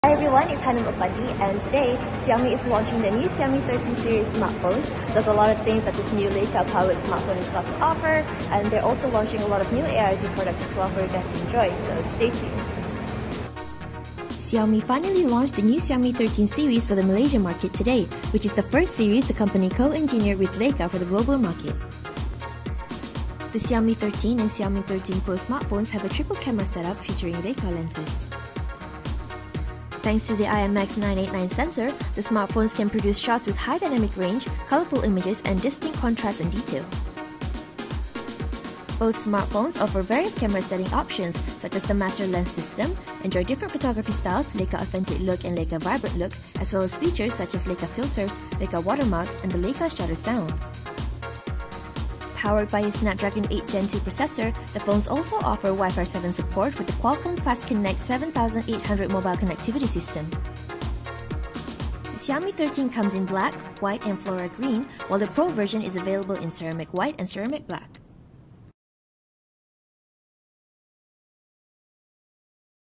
New Xiaomi 13 and Xiaomi 13 Pro
Xiaomi introduces new leica-powered smartphones that is powered by the Snapdragon 8 Gen 2 processor